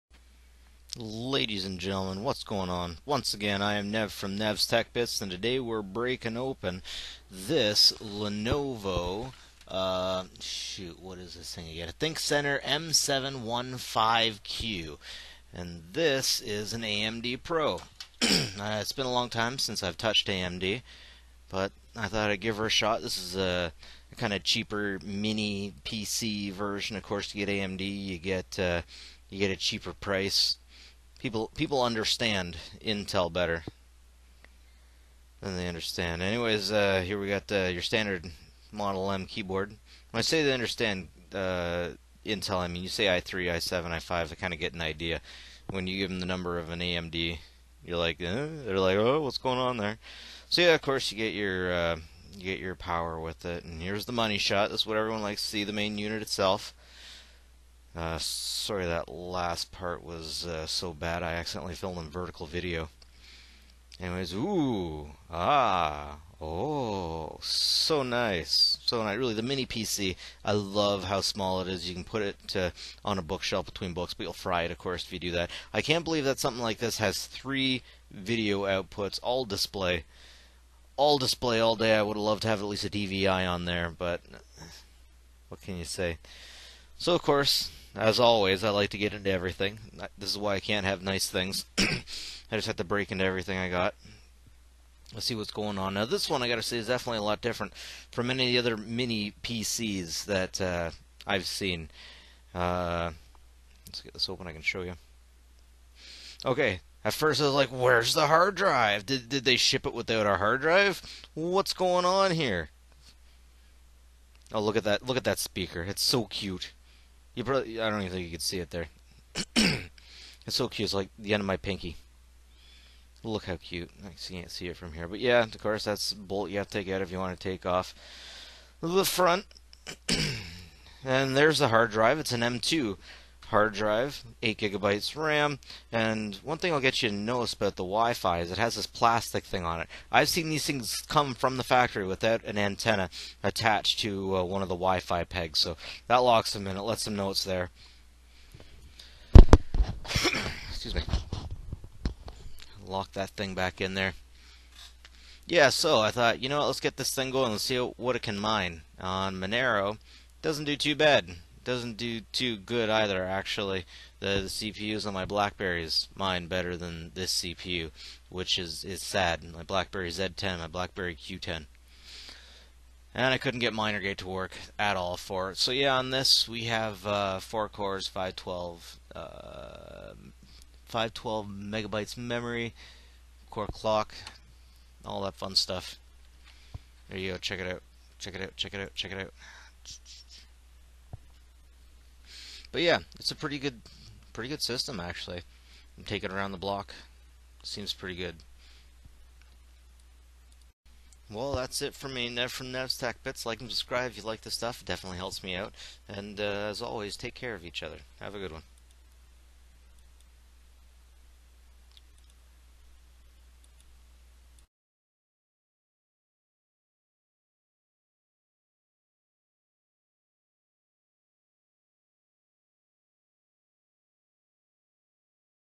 ThinkCentre M715Q AMD Pro Mini PC A12 9800E 10M3-0009US Unboxing, Teardown, Review, And Benchmark
ThinkCentre M715Q AMD Pro Mini PC 10M3-0009US Unboxing, Teardown, Review, And Benchmark

AMD PRO A12-9800E
Description: R7, 12 COMPUTE CORES 4C+8G
Class: Desktop
Socket: AM4
Clockspeed: 3.1 GHz
Turbo Speed: 3.8 GHz
No of Cores: 4
Typical TDP: 35 W
Other names: AMD PRO A12-9800E R7, 12 COMPUTE CORES 4C+8G
CPU First Seen on Charts:  Q1 2017
CPUmark/$Price:  28.22
Overall Rank:  959
Last Price Change:  $169.00 USD (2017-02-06)

ThinkCentre M715 TinyPDFExcel Print
Model 10M30009US
Product ThinkCentre M715q Tiny
Region US
Machine Type 10M3
TopSeller TopSeller
Processor AMD PRO A12-9800E (4C, 3.1 / 3.8GHz, 2MB)
vPro No
Graphics Integrated AMD Radeon R7 Graphics
Monitor Cable None
Memory 8GBx1
Storage 128GB SSD M.2 PCIe NVMe
External Optical / I/O Box None
WLAN + Bluetooth RTL8821AE ac, 1x1 + BT4.0
Optional Rear Ports 3rd DP
Speaker 1Wx1
Adapter Cage None
Toolless Chassis Screw None
Dust Filter None
Keyboard / Mouse USB / USB
VESA Mount None
Stand Floor Stand
Power Supply 65W 88% Adapter
Operating System Windows 10 Pro 64
Warranty 3-year, Onsite
Global No
End of Support Oct 25, 2021
Ann Date (mm/yy) 10/16